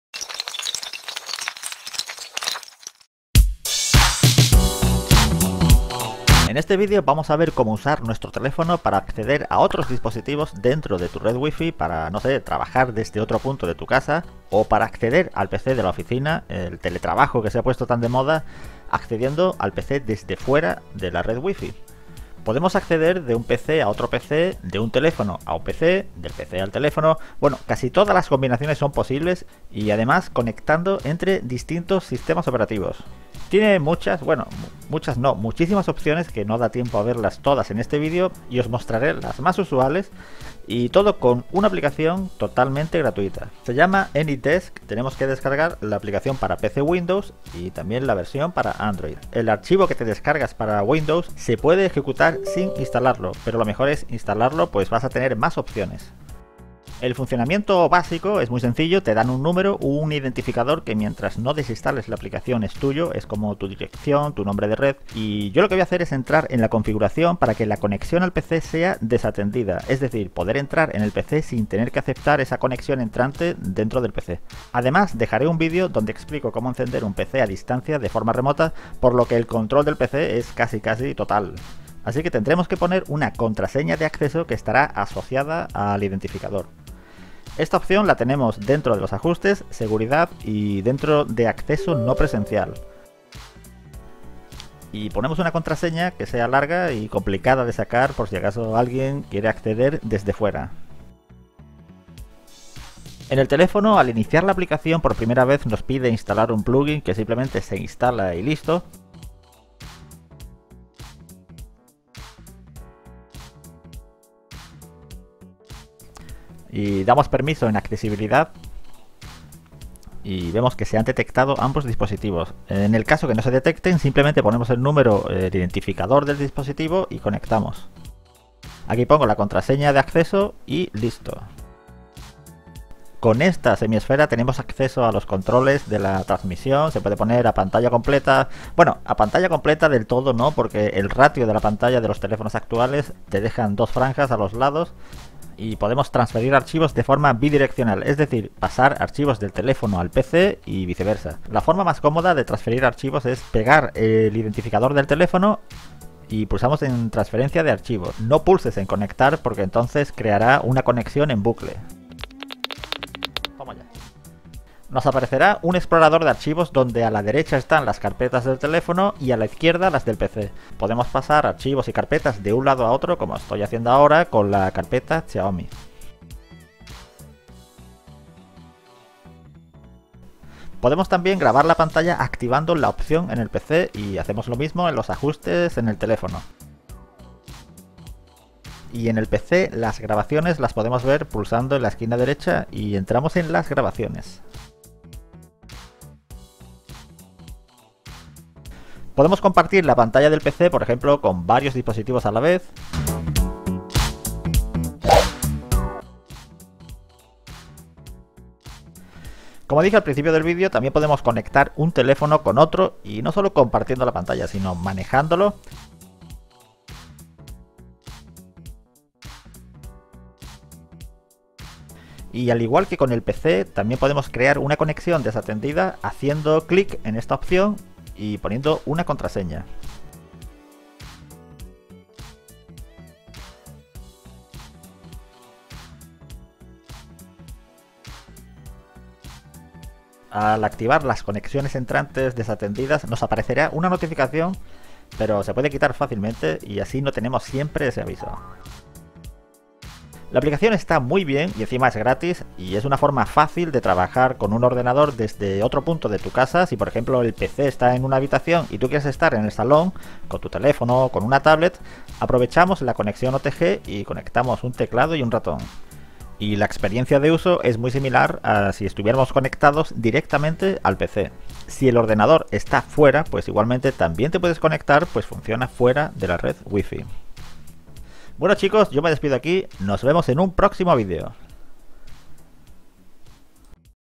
Accede de forma Remota desde Android a otro teléfono Android y PC Windows Mac
Aplicación multiplataforma para conectar de forma remota con otros dispositivos ,como  un PC Windows o Mac con un teléfono Android o conexión entre telefonos y tambien entre ordenadores con distintos sistemas operativos. Prácticamente todas las combinaciones son posibles

Cable OTG
Hub USB alimentado